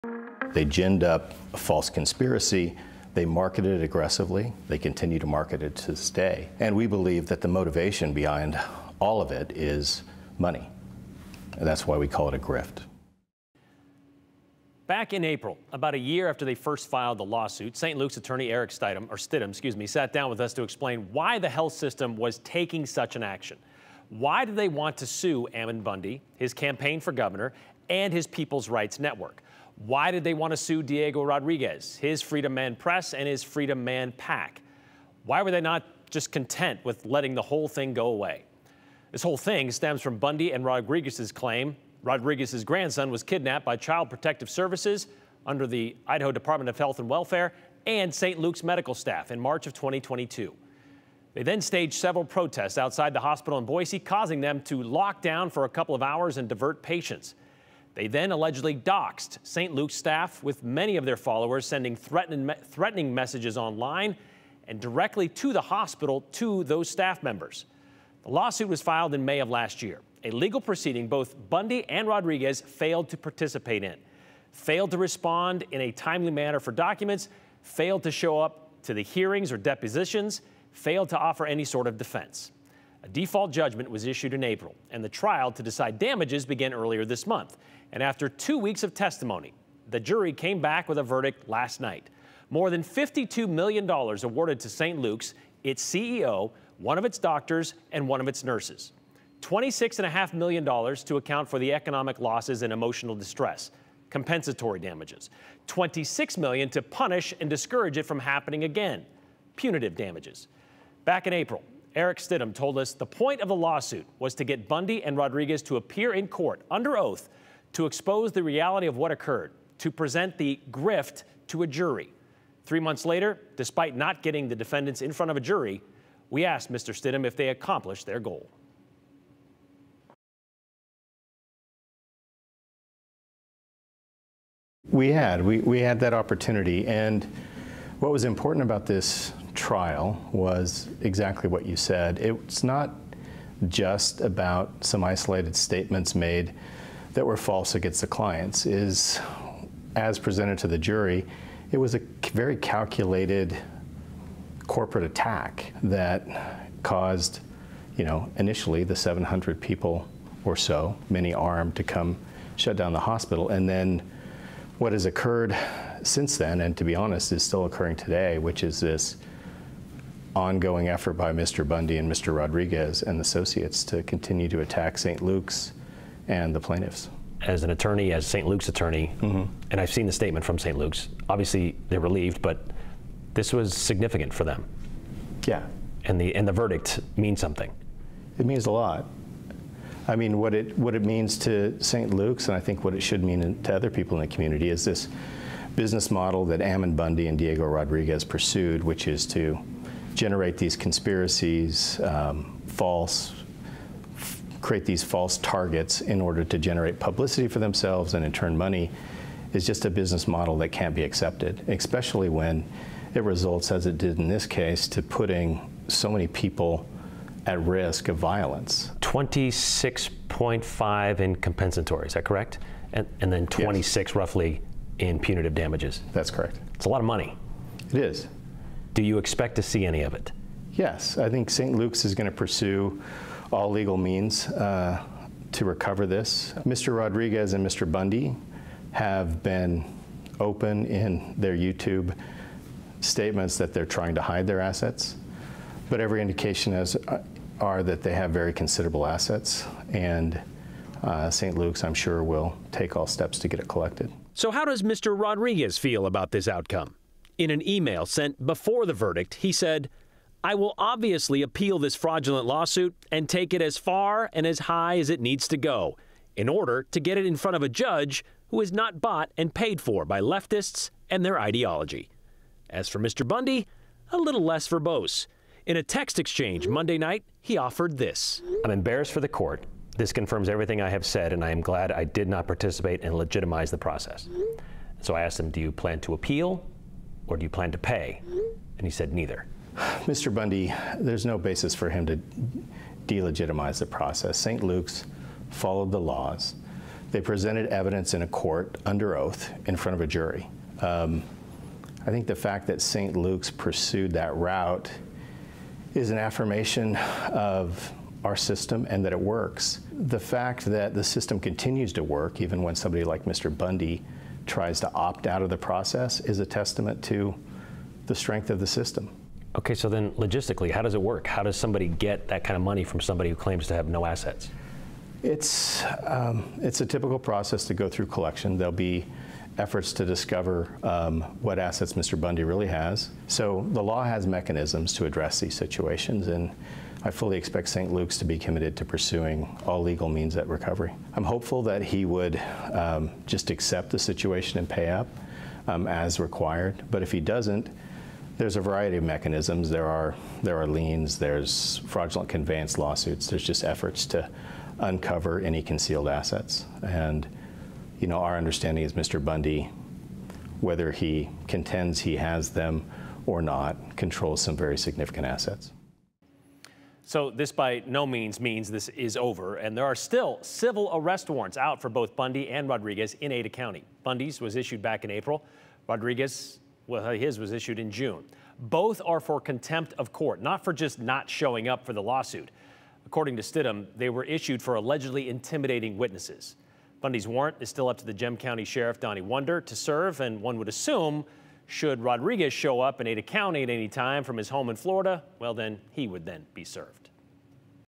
Bundy and associate Diego Rodriguez ordered to pay St. Luke's over $52 million in damages
St. Luke's attorney Erik Stidham discusses the case.